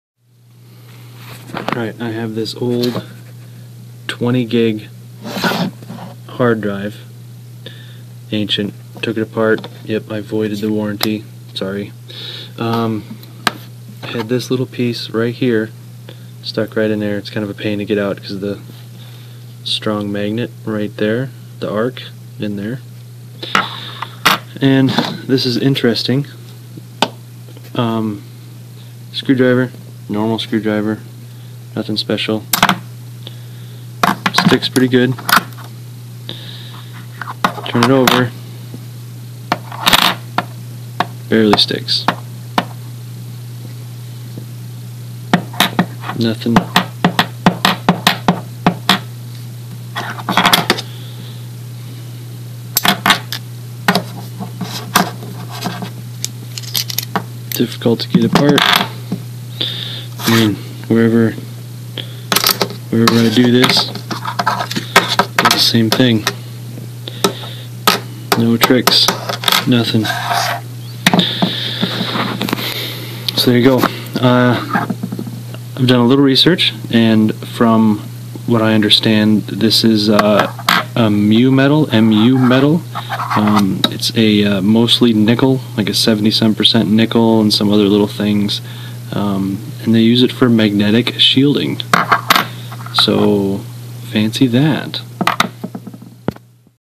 My introduction to MU metal
I saw a video that the guy took apart a hard drive and showed that fact the magnet was shielded on one side and not the other. So I said, I have spare hard drives laying around and took it apart and this is what I found.

The largest problem with "Magnet Motors" you find so prevalent on Youtube is the fact there is just as much pull one direction as there is the other. Meaning if you were to use a magnet to pull other magnets mounted to a wheel, once the magnets mounted on the wheel were to pass your main magnet the pull on the other side would pull it right back, known as the "Gate" or the "Sticky Spot."

If you can block the magnetic pull on the other side that may allow you to keep spinning the wheel. Once you can spin a wheel indefinitely, you can drive an electric generator with it.